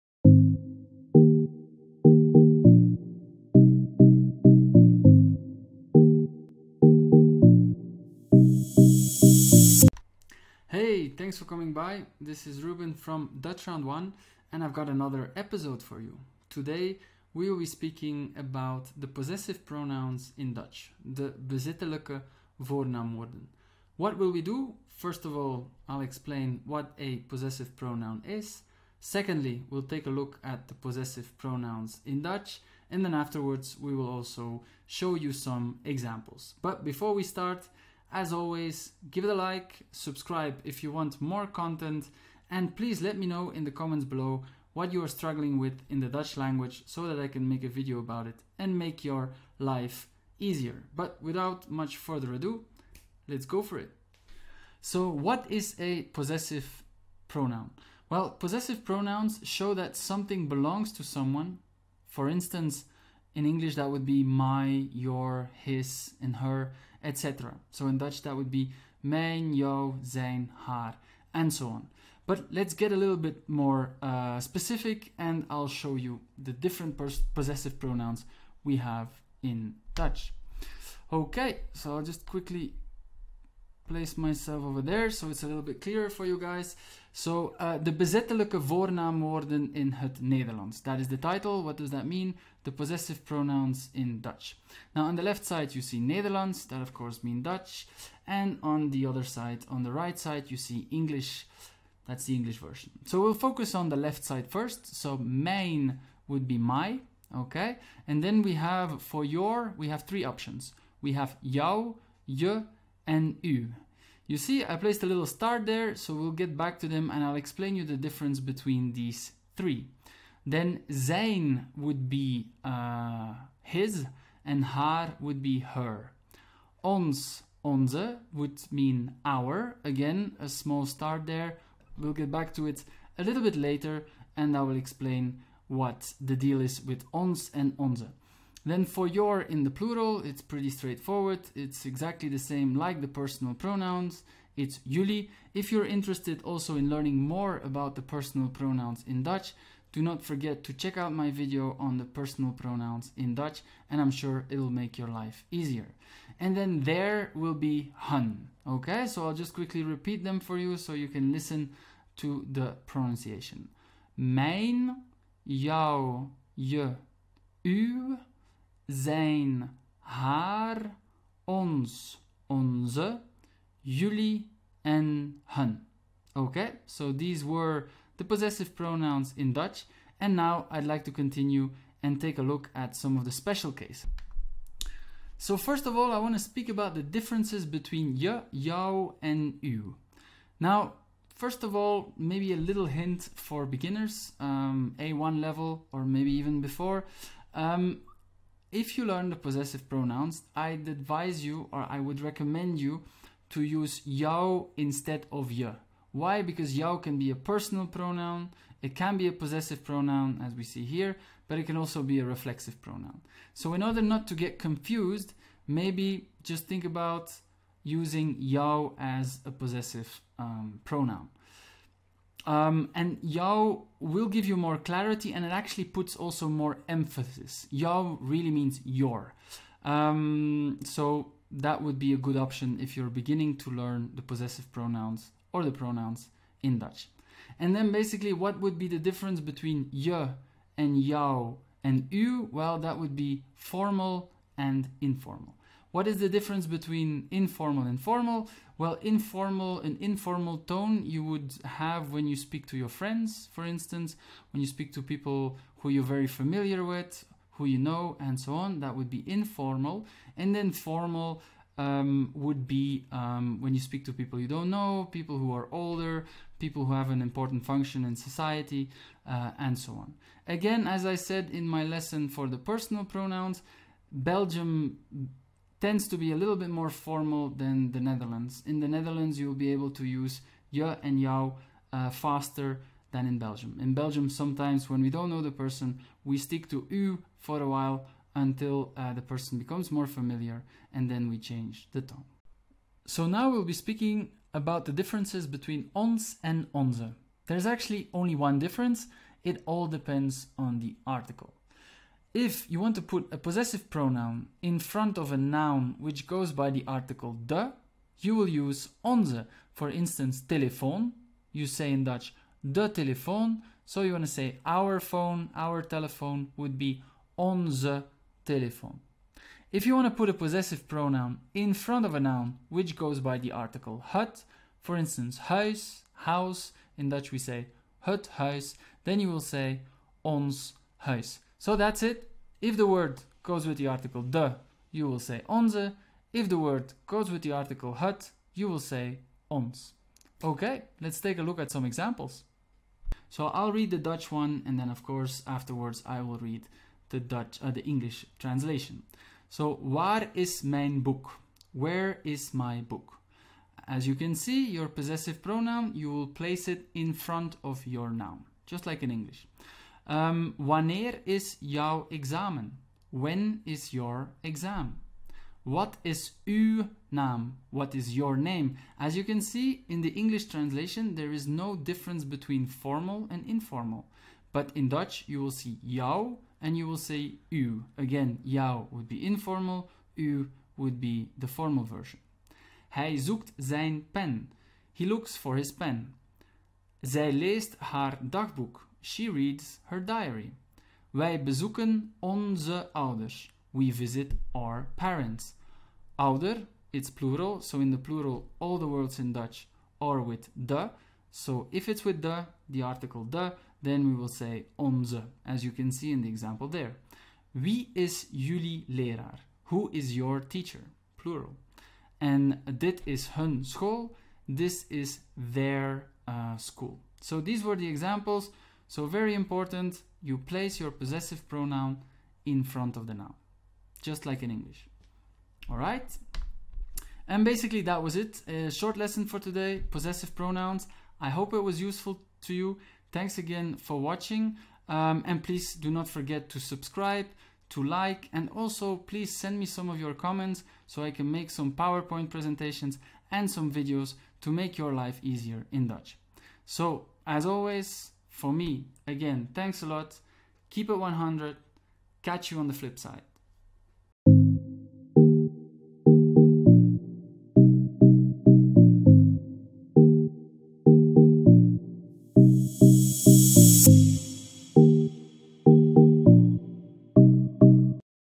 Possessive Pronouns in the Dutch language
In this video we will focus on the possessive pronouns. After a short definition, the actual pronouns and some examples you will be able to use them in your daily speech. As always I have tried to make it as straightforward as possible...

0:50 : What is a possessive pronoun ?
1:20 : The possessive pronouns in Dutch
3:02 : differences between je/jouw and uw
4:52 : difference between ons and onze
5:45 : examples
7:07 : outro

If you want to find out what my students think about me, please click on the

Bedankt en tot ziens...

Dutch Round 1